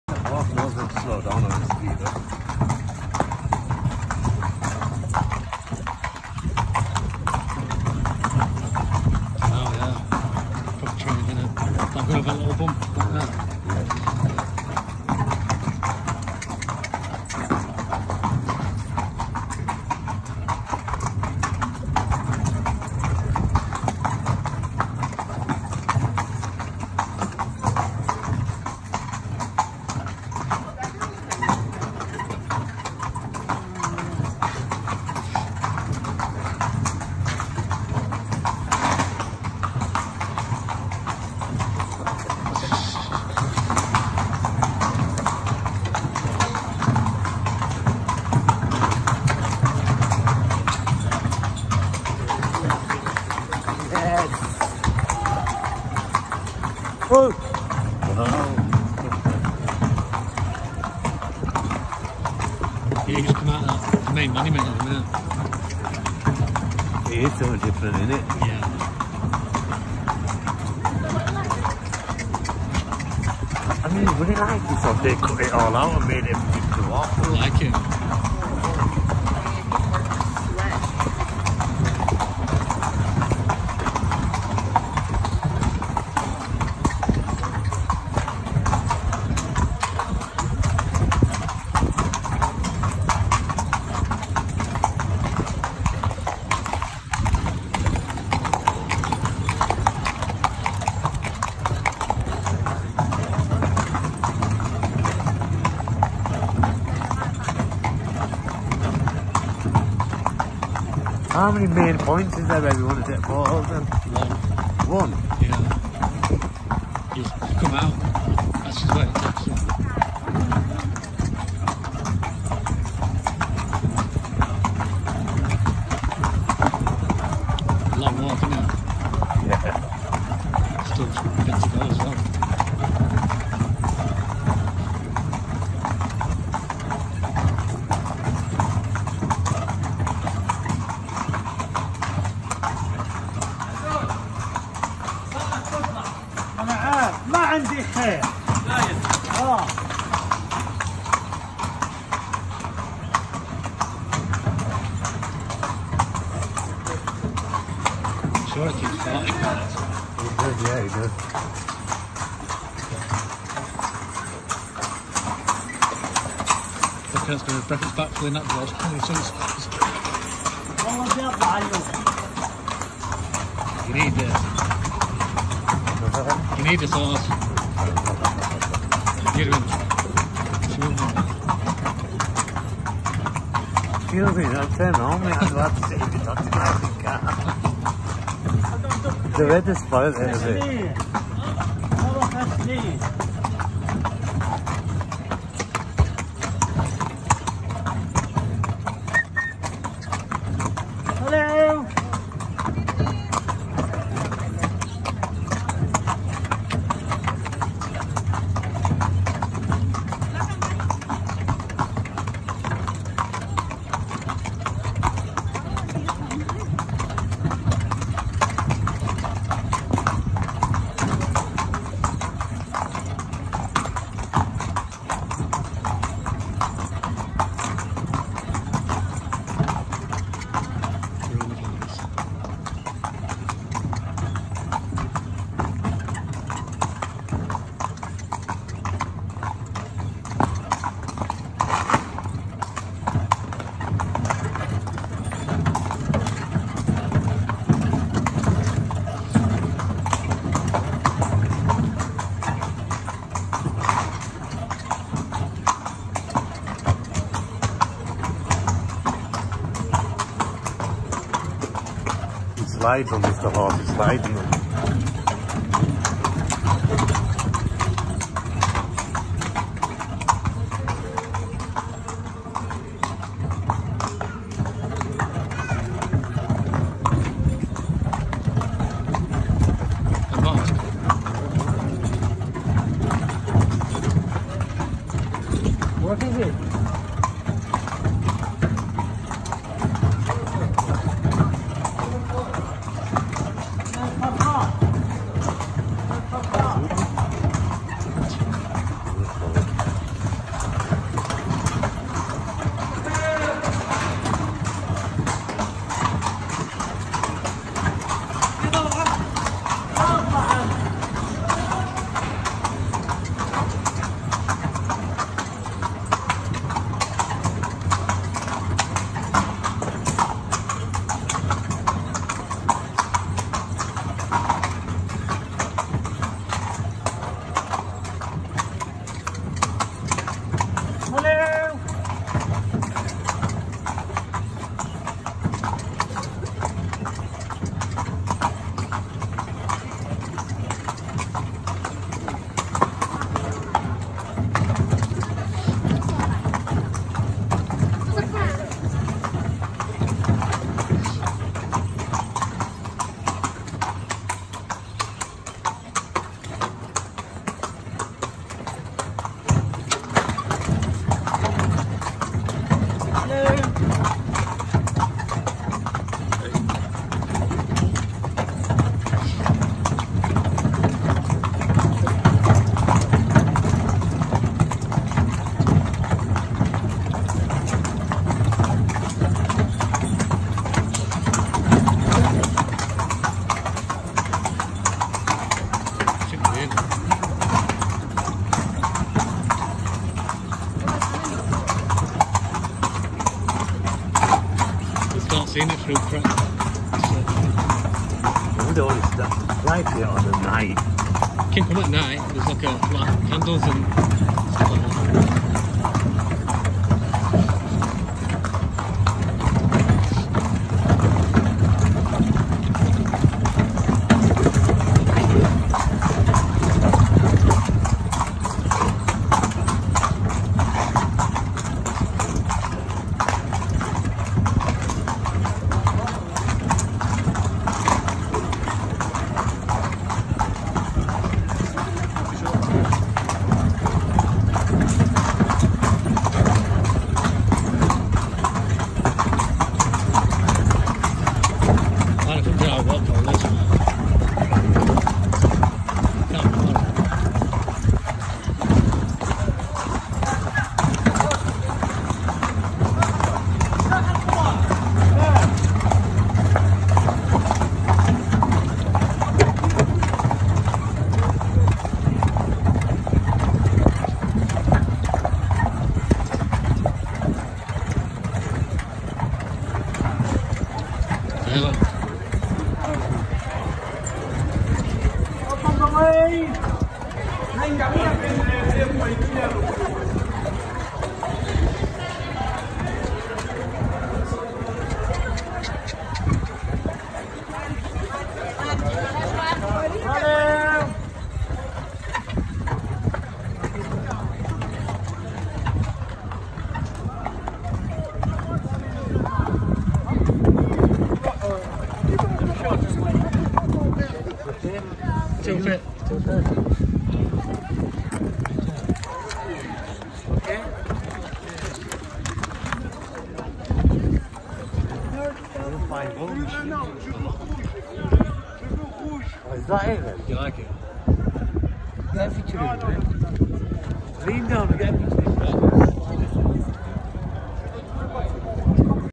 Horse ride to Petra - Petra Jordan - first view of the Treasury 🇯🇴
Horse and carriage ride down the siq at Petra to the treasury

This was my 5th wonder just got the Taj Mahal and Machu Pichu to visit